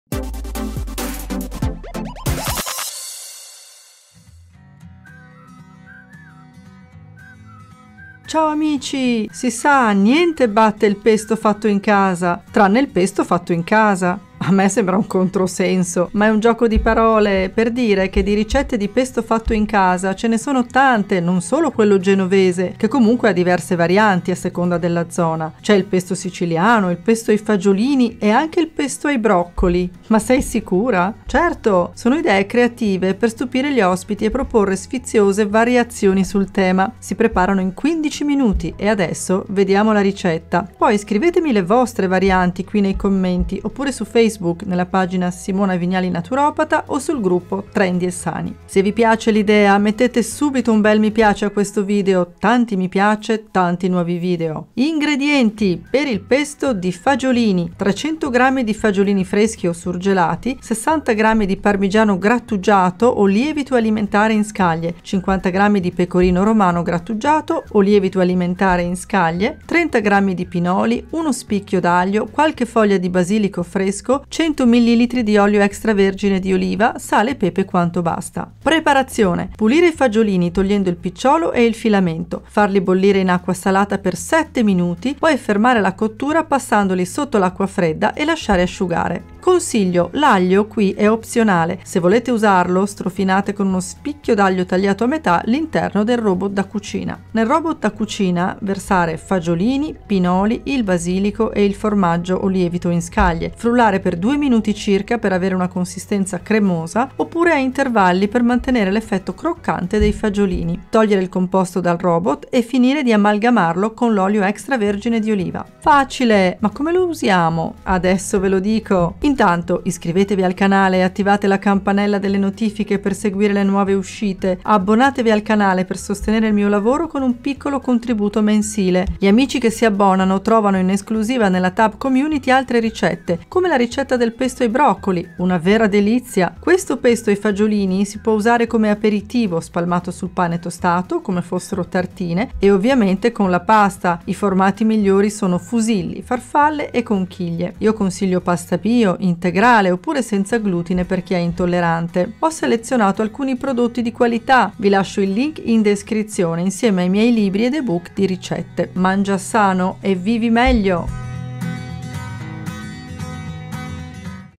RICETTA PESTO CREATIVO fatto in casa con FAGIOLINI e PINOLI facilissimo e velocissimo!
Ciao amici! Ecco una ricetta sfiziosa per far un pesto fatto in casa diverso, ma ugualmente goloso!

🌱 PASTA SENZA GLUTINE che uso e consiglio

Nota bene

Simona Vignali